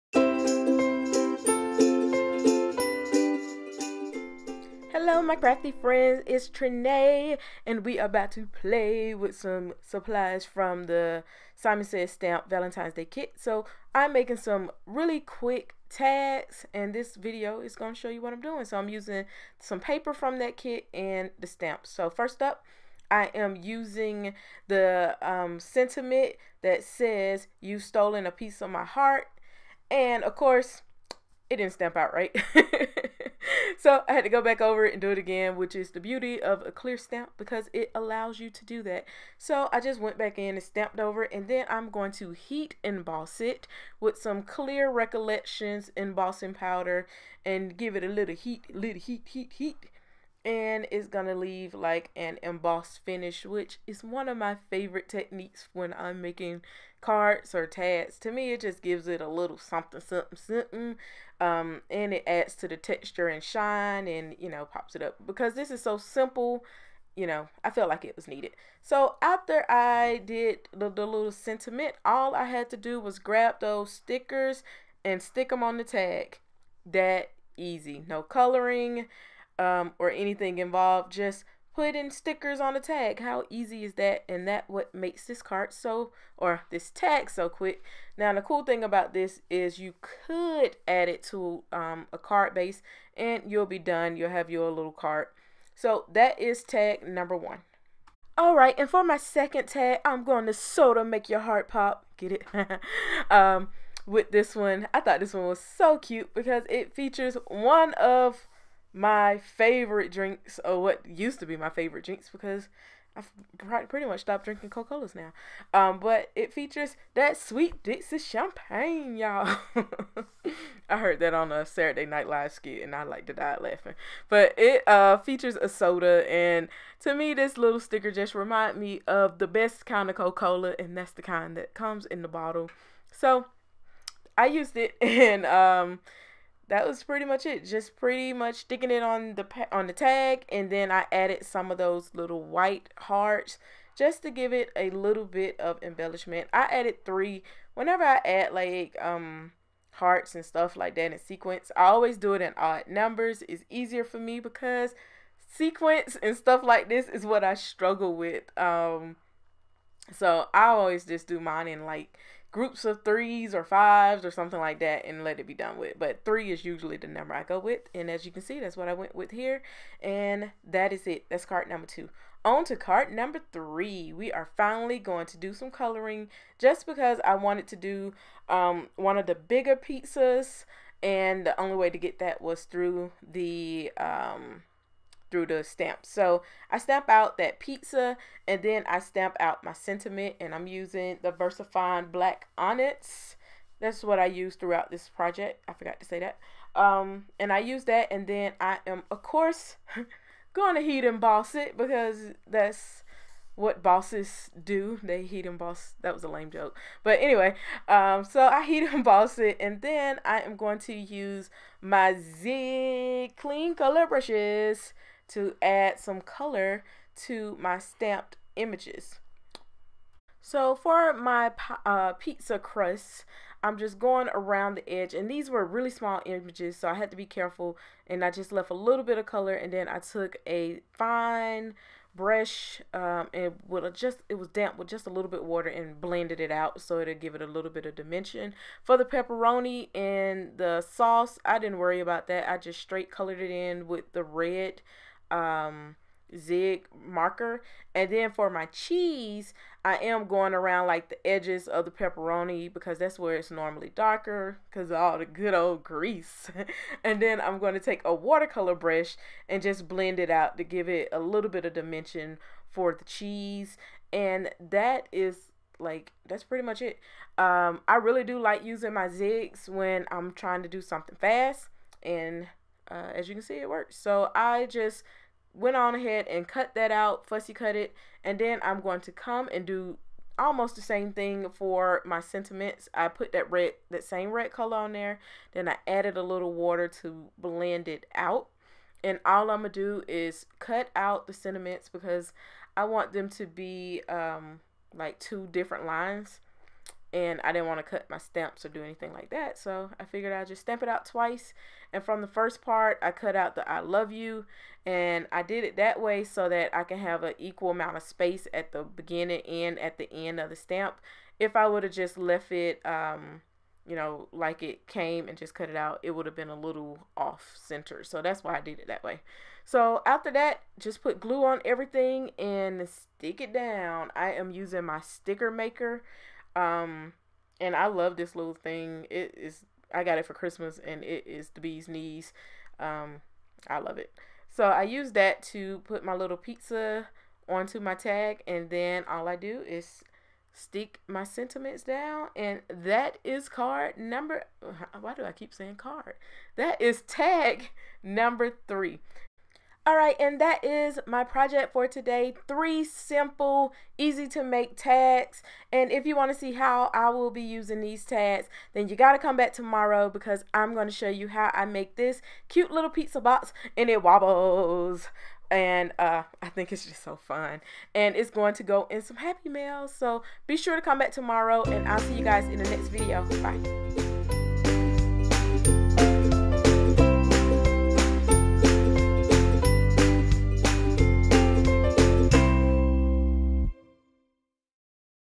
Clean and Simple Tags | Simon Says Stamp Valentine's Day Card Kit
PRODUCTS MENTIONED
Simon Says Stamp Valentine's Day Card Kit
Zig Clean Color Real Brush Set

FILMING
Camera: Canon 80D 10-18mm
Editing: iMovie